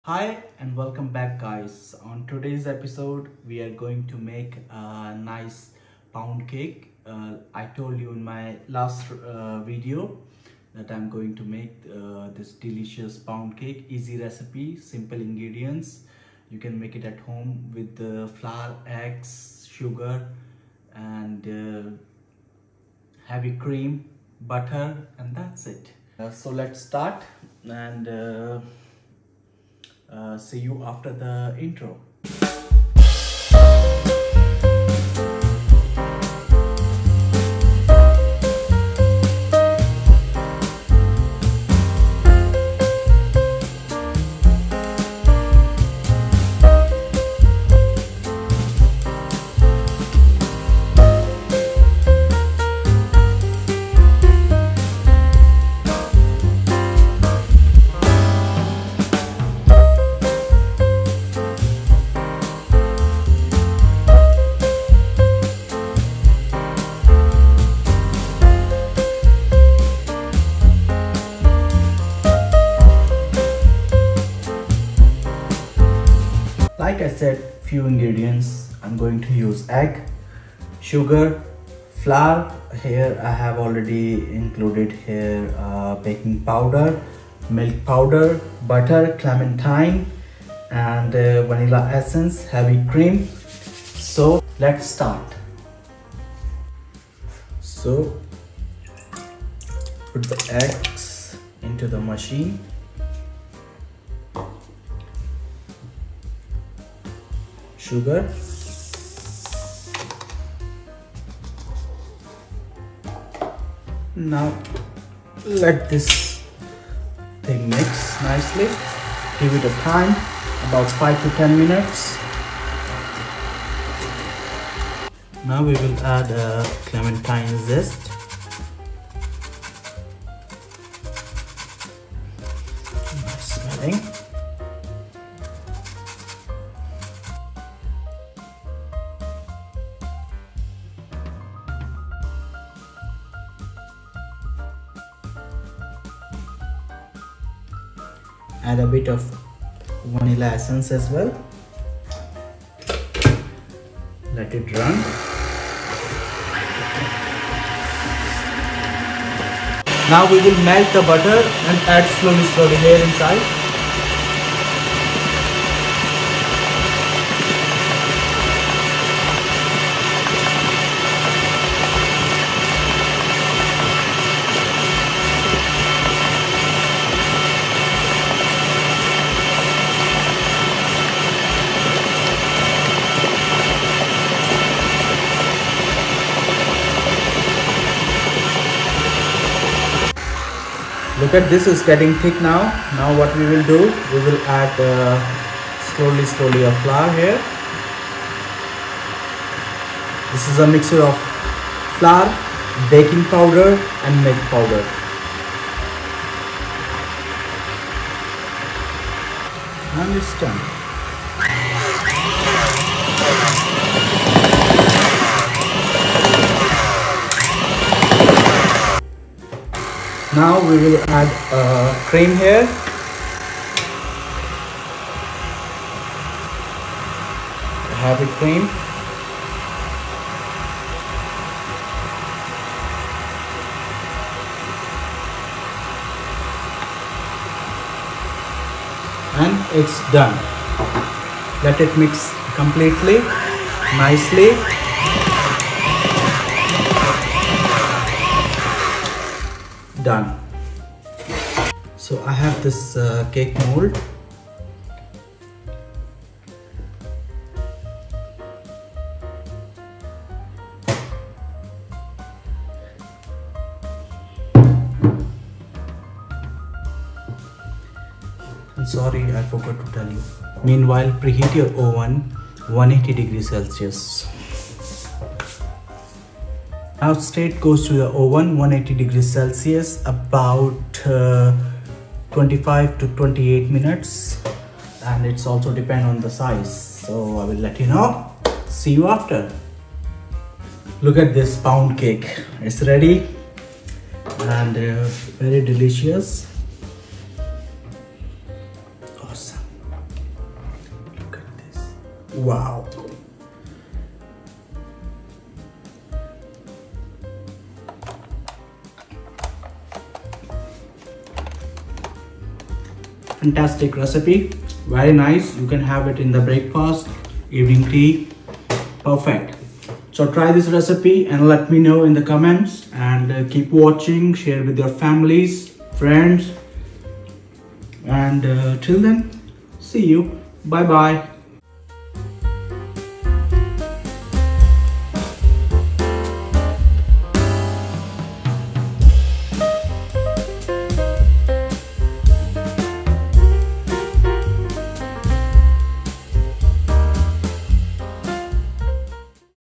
PERFECT POUND CAKE | EASY RECIPE BY CHEF QURESHI
Pound cake is a true melt- in-the-mouth treat! Made of just few simple ingredients but yet very rich and buttery flavor makes it a perfect breakfast or afternoon tea companion ! If you think that your cake is not sweet enough you can serve it with some fruit marmalade or honey.

                                  POUND CAKE

 EGGS                                   420 GM
 SUGAR                                   400 GM
 CLEMENTINE  ZEST           2 TBSP
 VANILLA ESSENCE           1 TSP
 MELTED BUTTER                   450 GM
 FLOUR                                   370 GM
 MILK POWDER                   30 GM
 BAKING POWDER           11 GM
 HEAVY CREAM                   130 GM